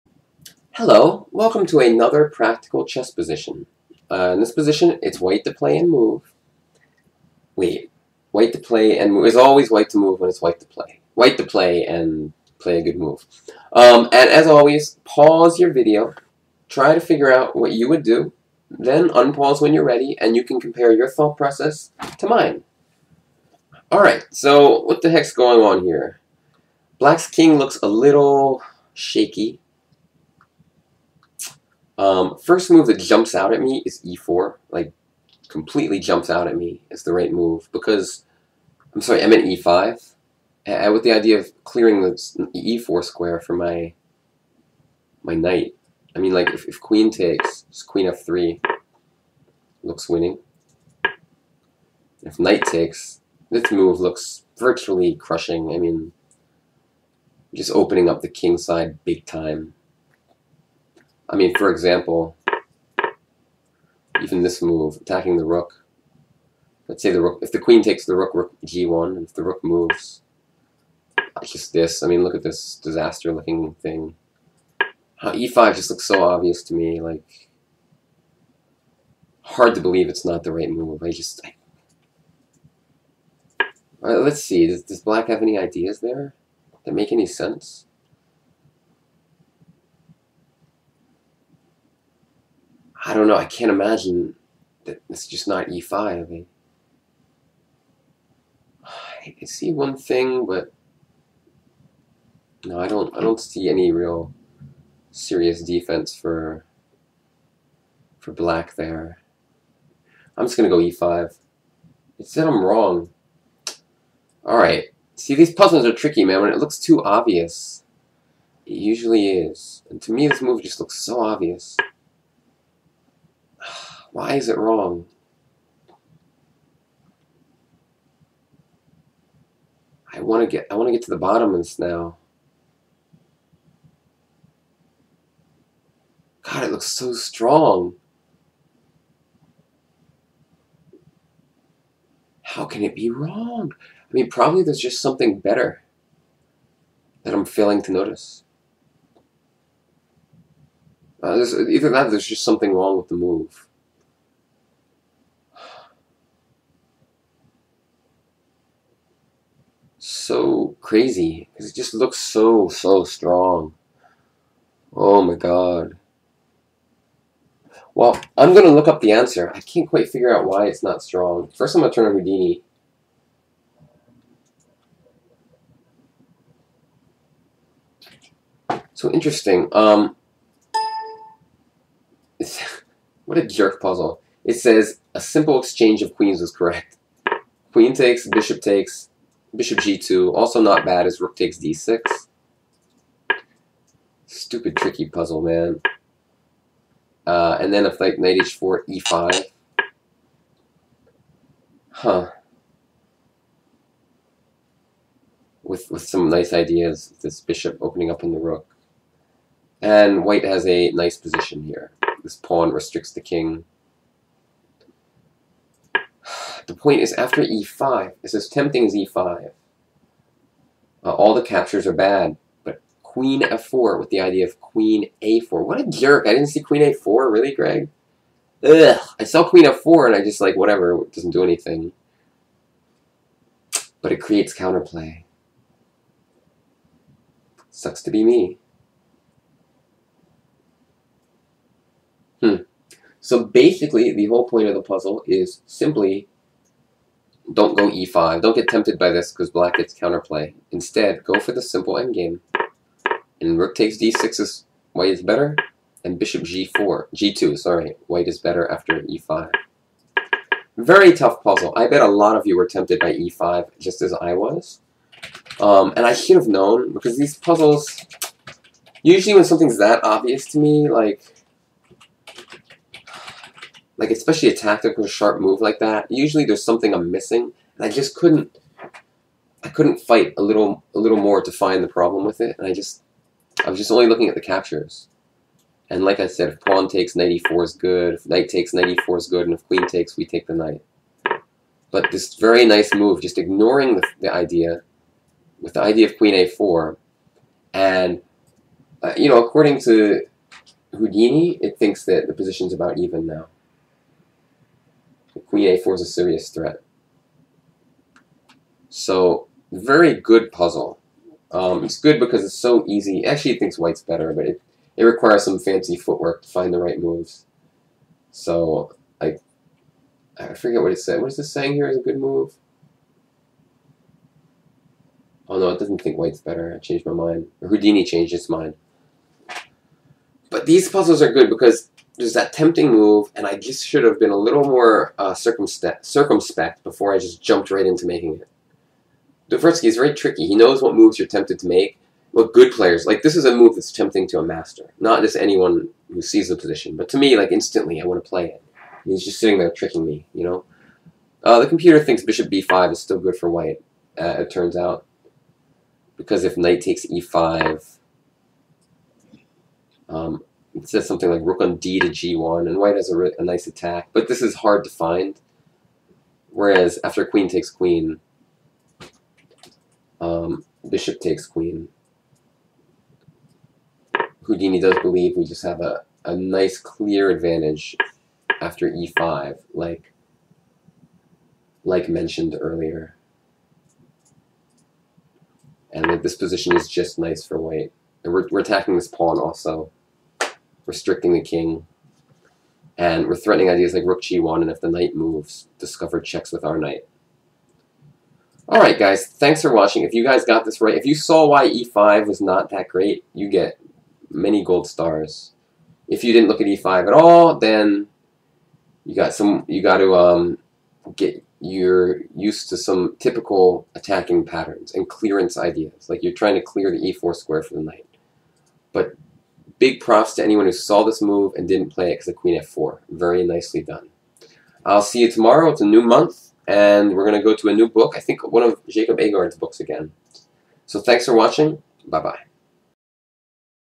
Practical Position #84
Thanks to Mark Dvoretsky for this puzzle. I highly recommend that everyone buy some of his books. They are the absolute best for players rated above 2000